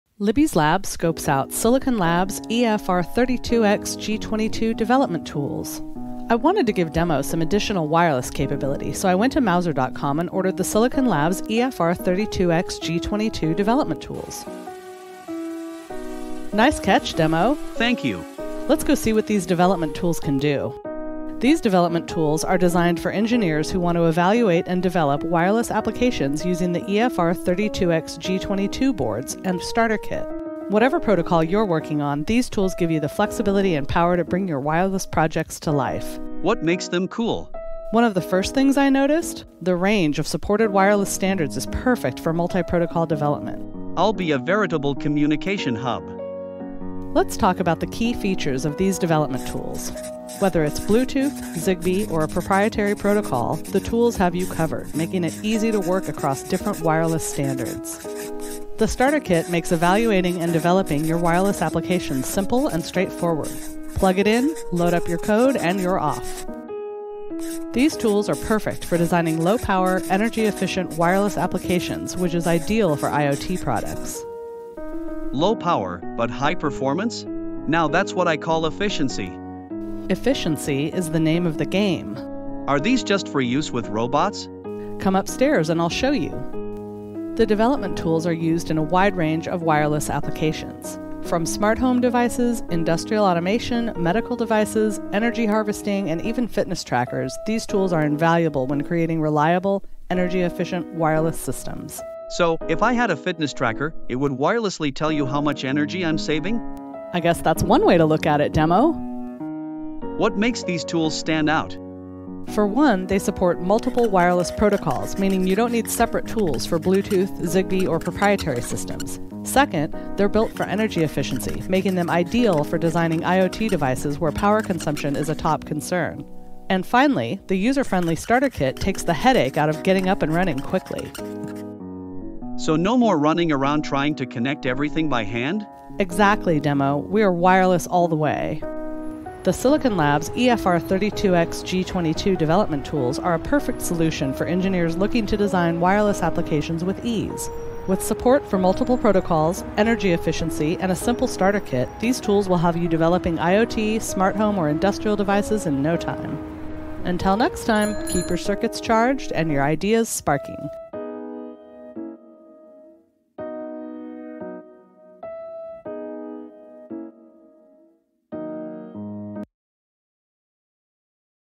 Libby's Lab - Scopes Out Silicon Labs EFRxG22 Development Tools | Mouser Electronics
Join Libby in this episode of “Libby’s Lab” as she explores the Silicon Labs EFR32xG22 Development Tools, available at Mouser.com! These versatile tools are perfect for engineers developing wireless applications with Bluetooth®, Zigbee®, or proprietary protocols. Designed for energy efficiency and ease of use, the starter kit simplifies development for IoT, smart home, and industrial devices. From low-power IoT projects to fitness trackers and medical devices, these tools offer multi-protocol support, reliable performance, and hassle-free setup. Watch as Libby and Demo dive into how these tools can bring wireless projects to life. Keep your circuits charged and your ideas sparking!